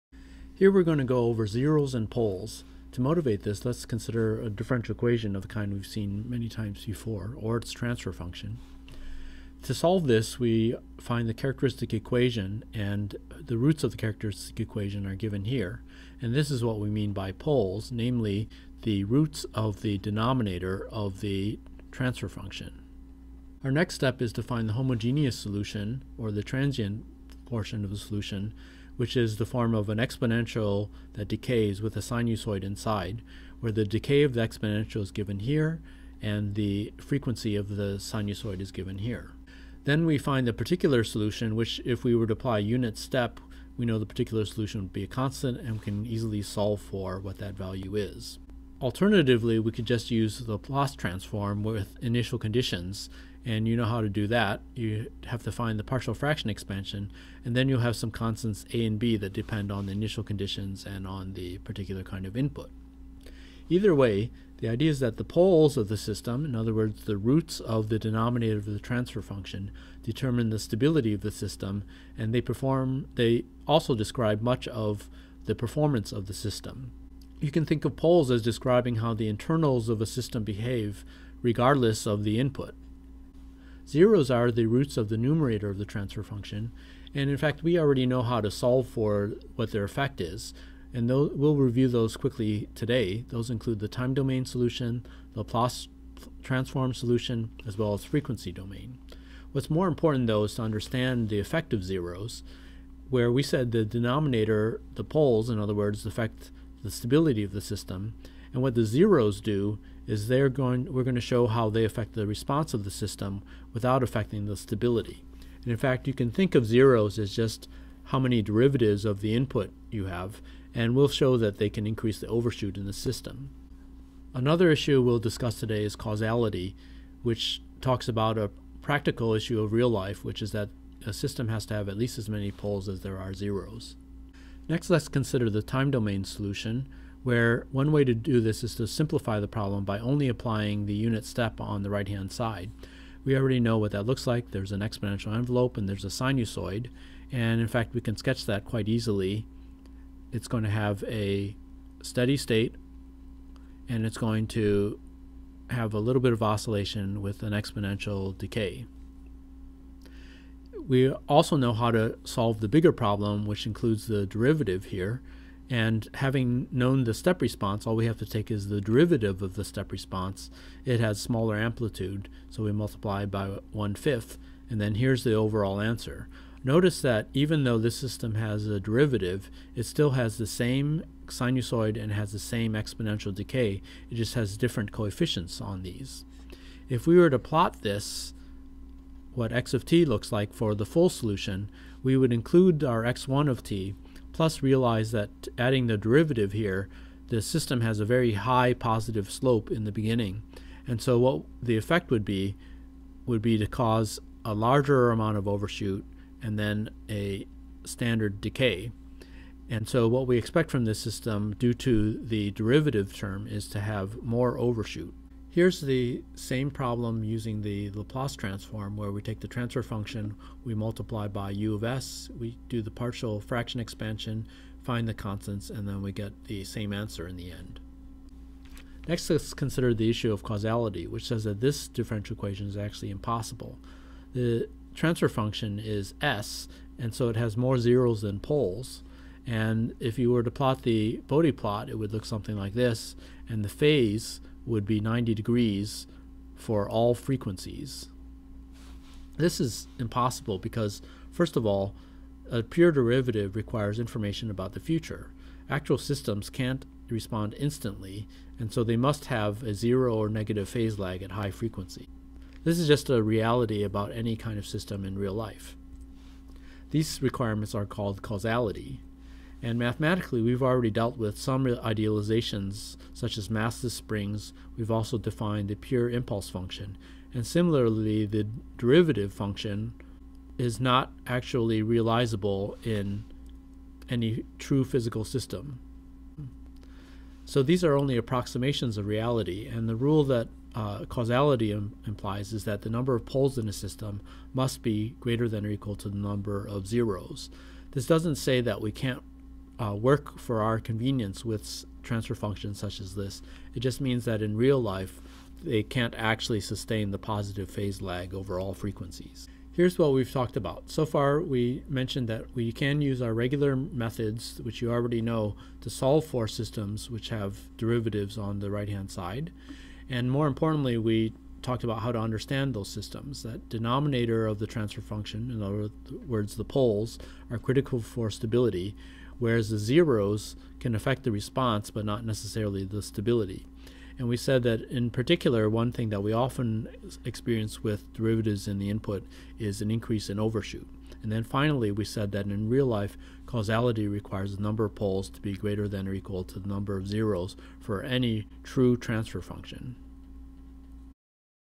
Lecture 26 Poles and zeroes
Video supplementary lectures from "Modeling, Analysis, and Control of Dynamic Systems," ME 360 Winter 2015. Supplementary video lectures for "Modeling, Analysis, and Control of Dynamic Systems," ME 360 Winter 2015, at University of Michigan.